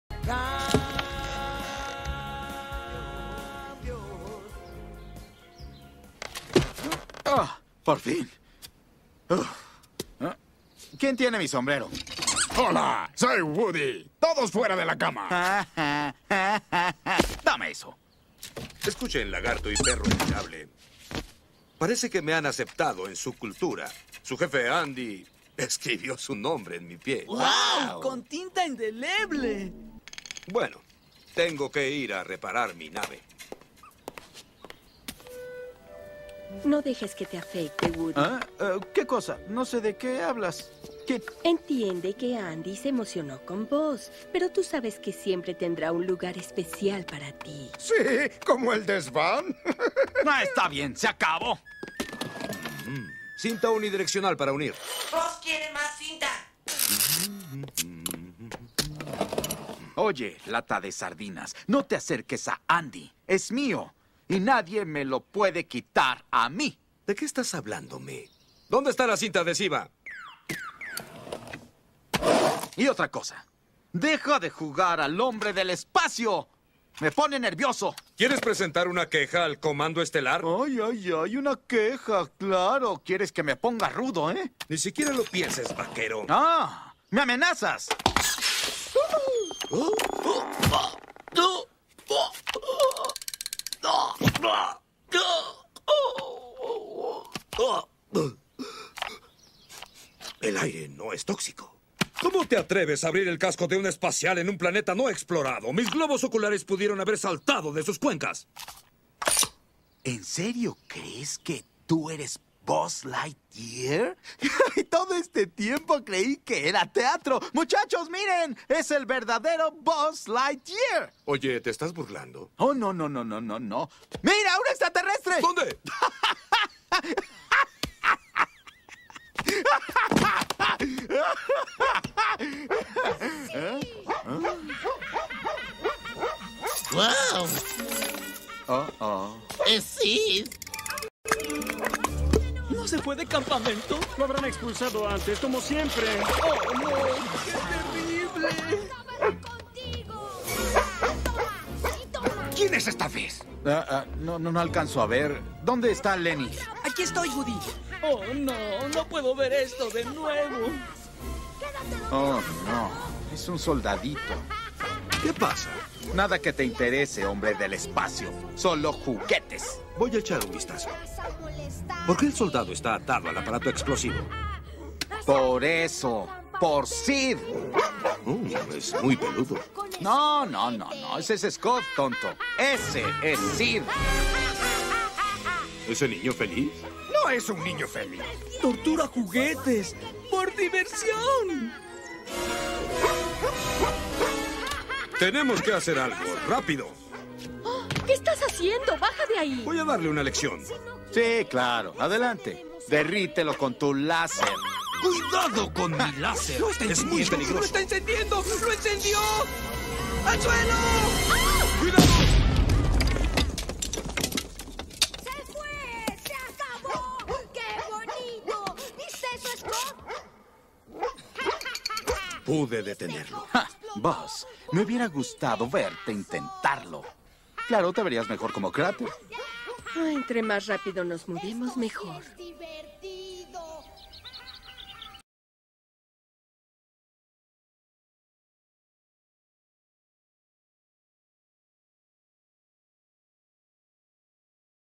Equestria Story La Película (1995) "La Discusión entre C.R Y Heítronix / Kylum Mano Metálica Part 6
Mientras los días transcurrieron,C.R  Molesto decide confortarme por el desacuerdo del juguete  favorito, cuando un risa malvada se escucha descubriendo que un malvado villano ha hecho su acto de entrada.

Introduciendo Al 3ra Villano creado Heítronix Animation.